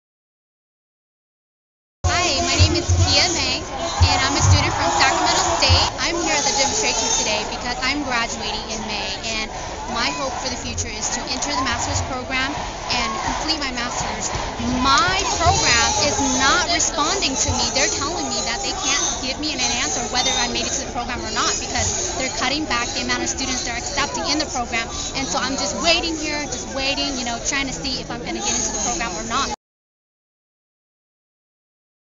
Students Speak Out at Sacramento's Protest
org Youth Radio covered protest held at Berkeley, Oakland, and Sacramento.